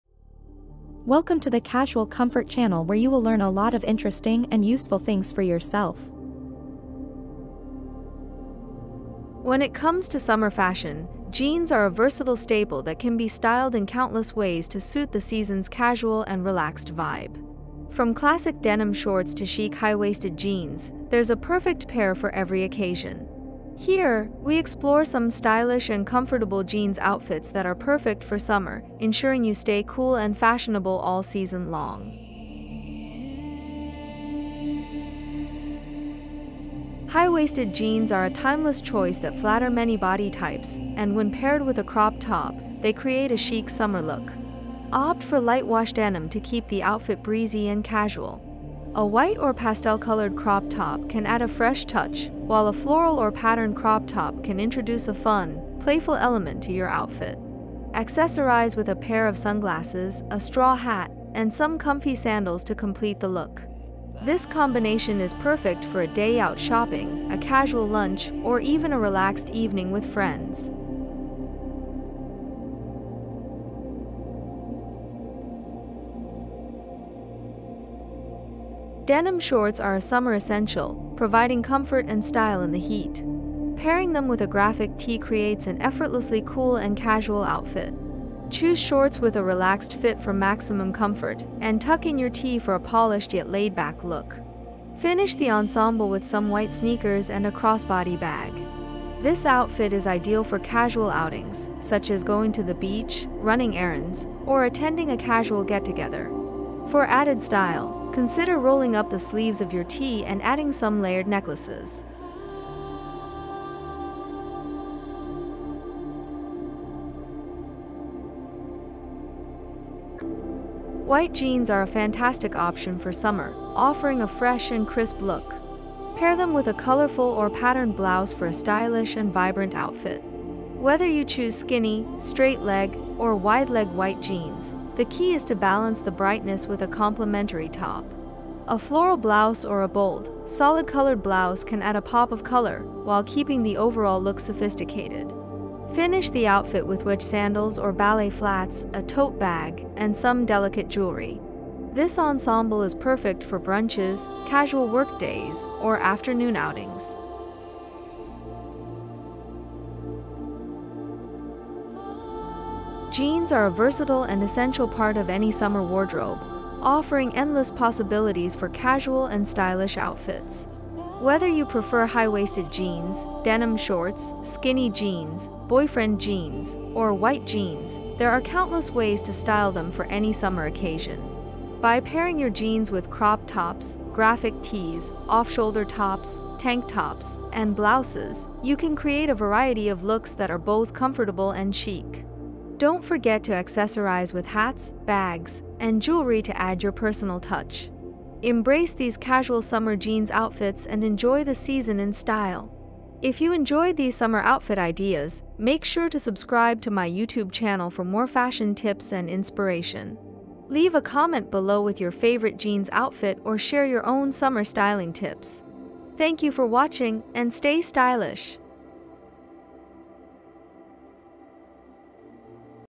Summer Casual Jeans Outfits for Women | Effortless and Trendy Looks | 2024 Fashion Trends
Stay stylish and comfortable this summer with our collection of casual jeans outfits for women. In this video, we showcase a variety of denim styles perfect for warm weather, including classic skinny jeans, relaxed boyfriend jeans, and chic high-waisted options. Discover how to pair your favorite jeans with breezy tops, lightweight jackets, and trendy accessories to create effortlessly chic summer looks. Whether you're heading out for a casual day or an evening gathering, these outfit ideas will keep you looking fresh and fashionable all season long.